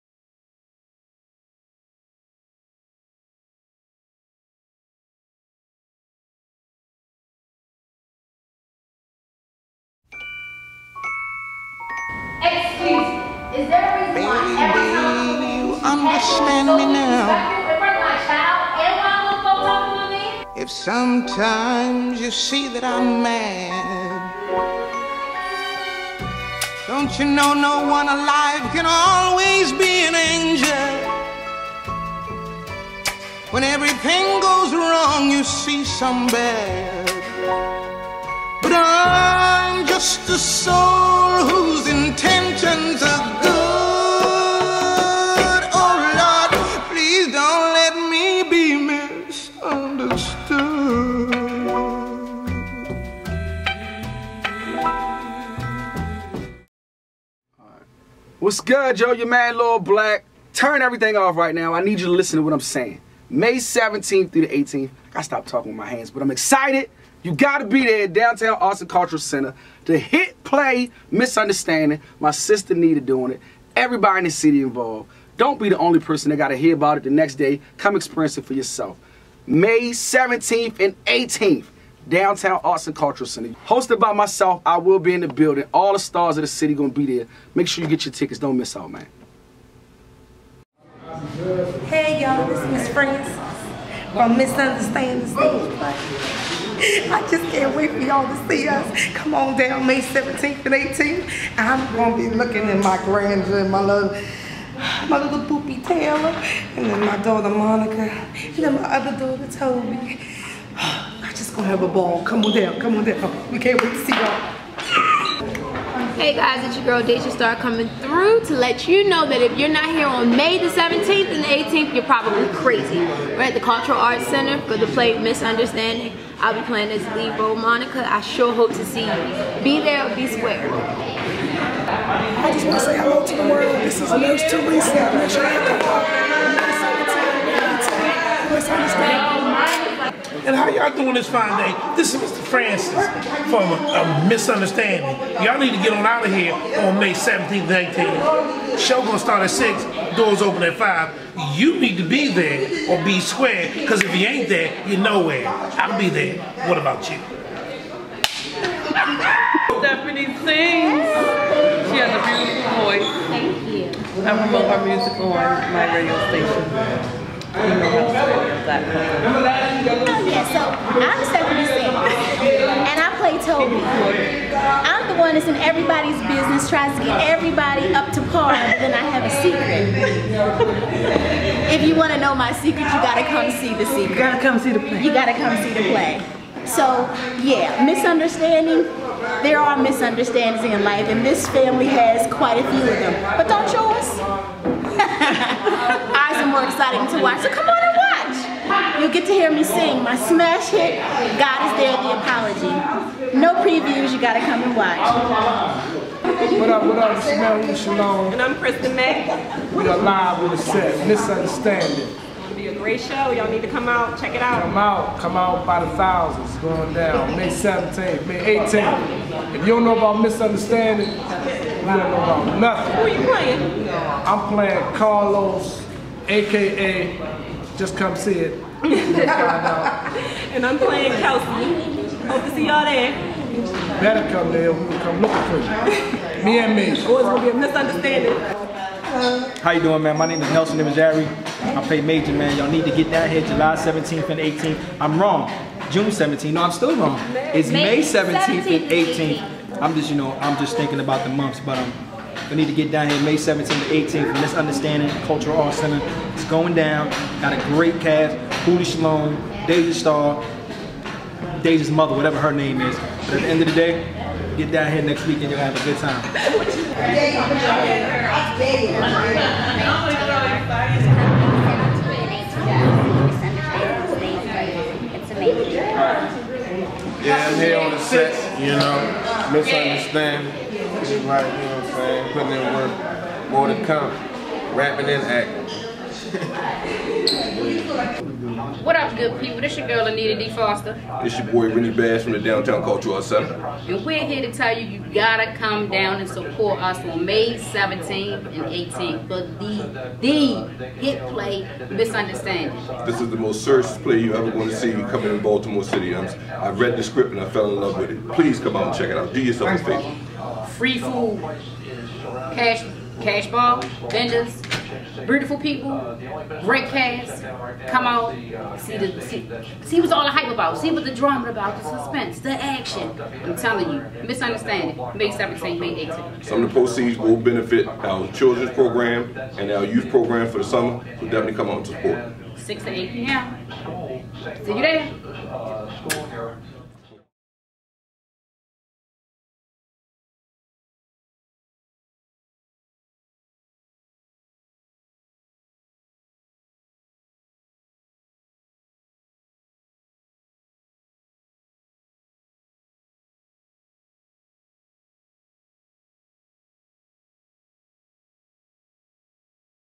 MISS-UNDERSTANDING The Stage Play- Promo
"In every family there is a mis-understanding"  Come watch The
Stage Play "Miss- Understanding"
Written, produced, and Directing by: Anita D. Foster
Being Me, LLC Productions

410-837-ARTS
401 North Howard St. Baltimore, Maryland 21201